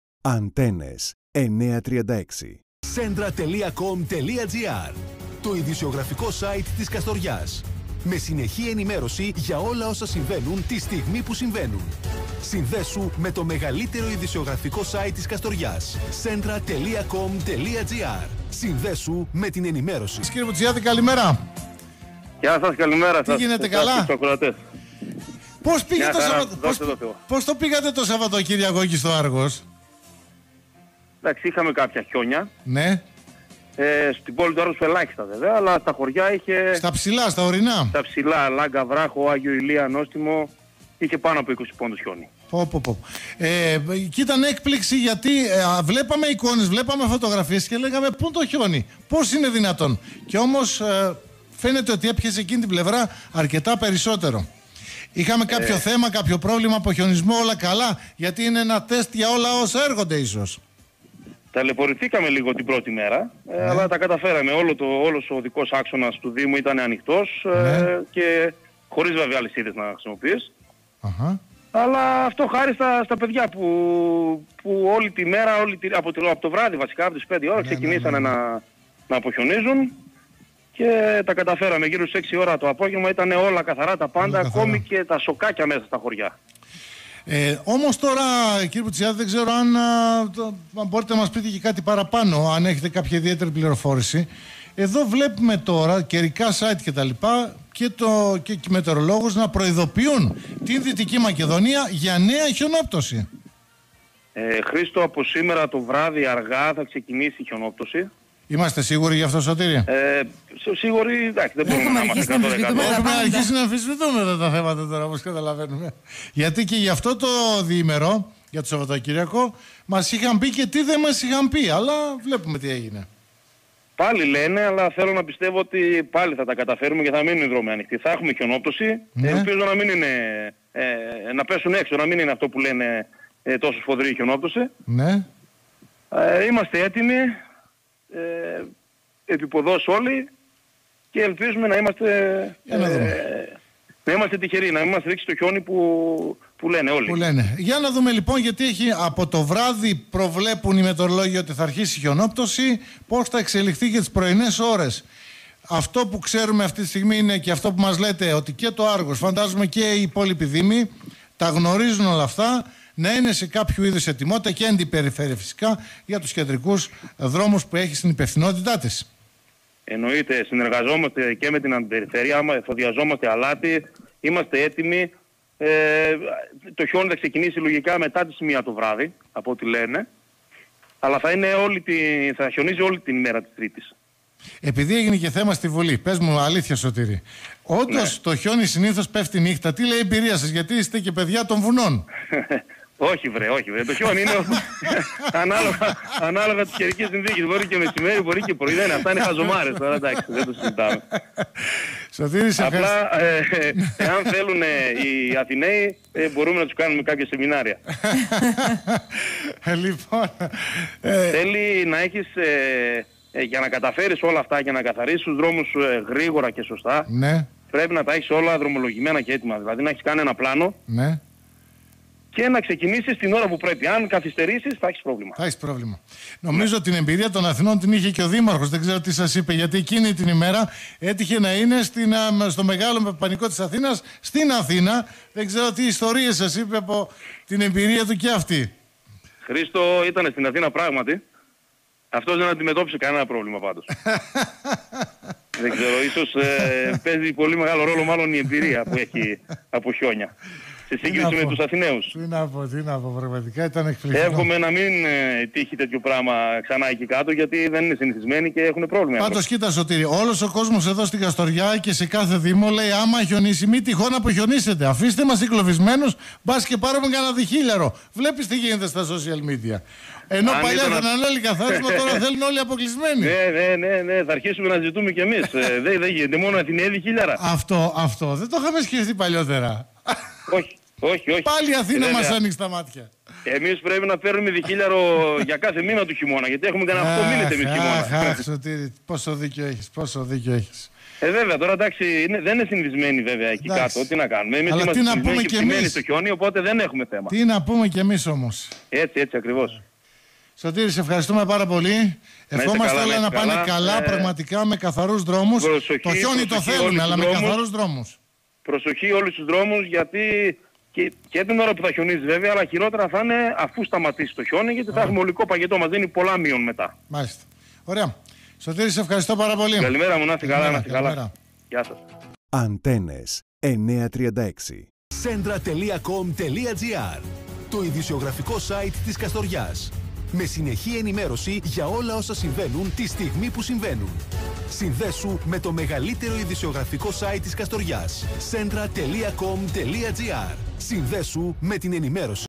Ο Σωτήρης Μπουτσιάδης για τα χιόνια του Σαββάτου και τον επερχόμενο χιονιά
Για την αντιμετώπιση της χιονόπτωσης του Σαββάτου στα χωριά του Άργους Ορεστικού αλλά και τον επερχόμενο χιονιά, μίλησε στον Χρήστο Νατσούλη και τον Antennes 93.6, ο αντιδήμαρχος Άργους Ορεστικού, Σωτήρης Μπουτσιάδης.